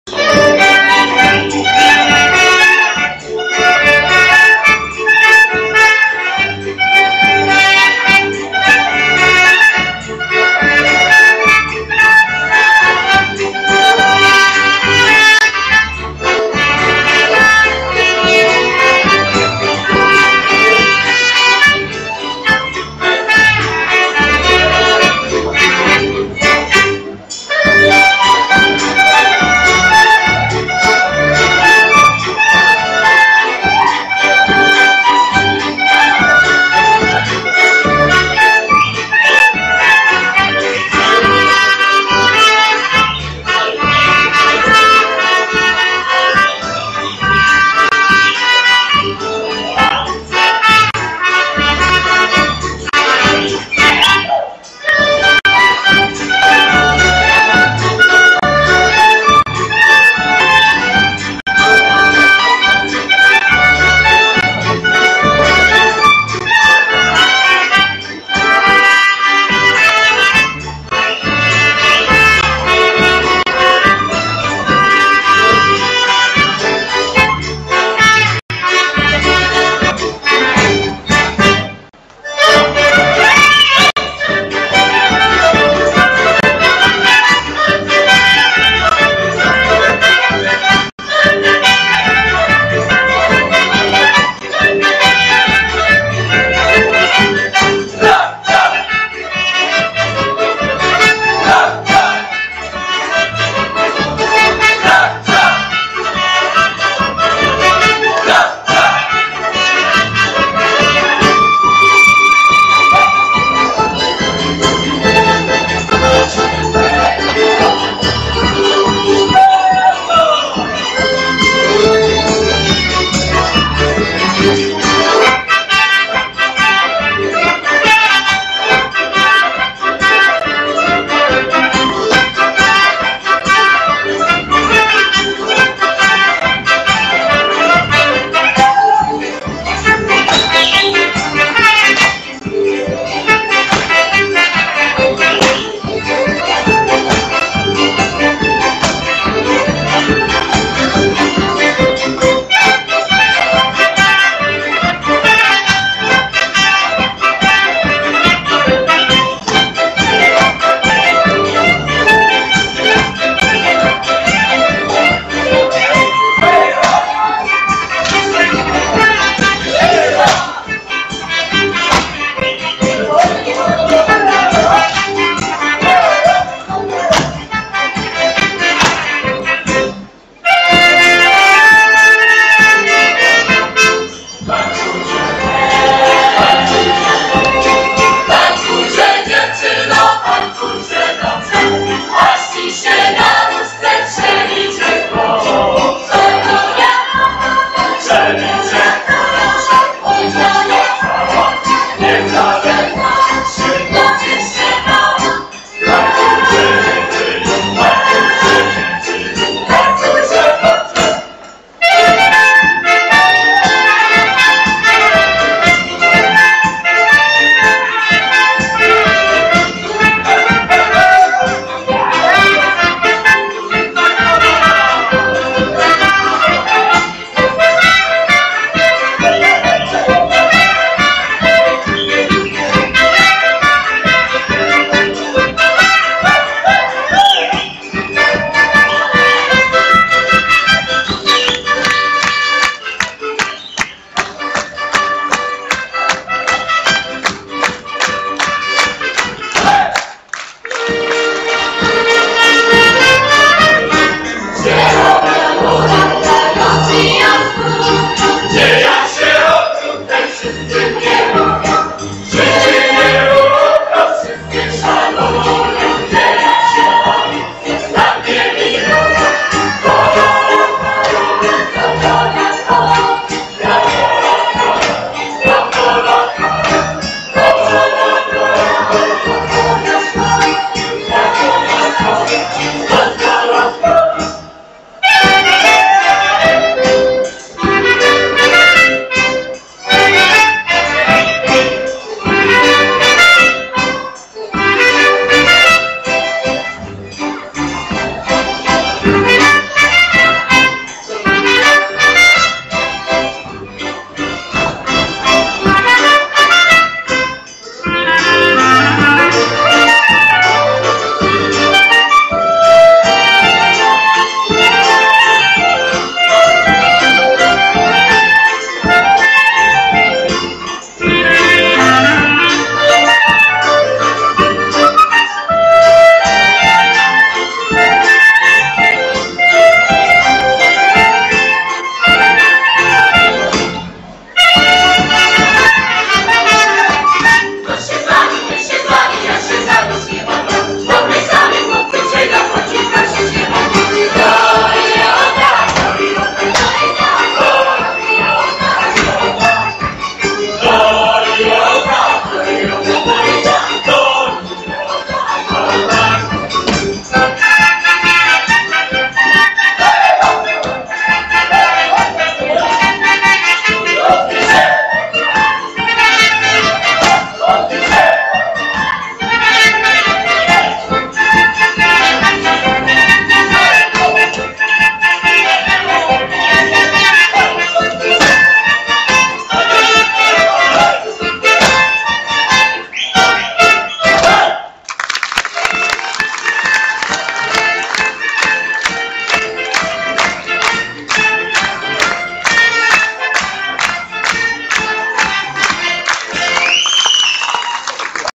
Tańce kurpiowskie 1993
Koncert z okazji 45 - lecia Zespołu Pieśni i Tańca Ziemi Lubelskiej im Wandy Kaniorowej, Lublin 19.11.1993r. sala widowiskowo - sportowa Technikum Kolejowego.
Wystąpili: II Reprezentacja